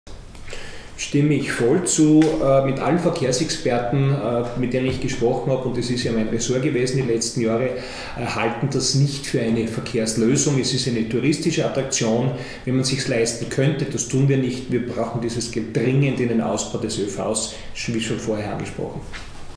10 - "Die Murgondel ist eine utopische Aktion..." (Mario Eustacchio, FPÖ)
10 - "Die Murgondel ist eine utopische Aktion, die keinen vorteil bringt und nur Geld kostet" (Mario Eustacchio, FPÖ)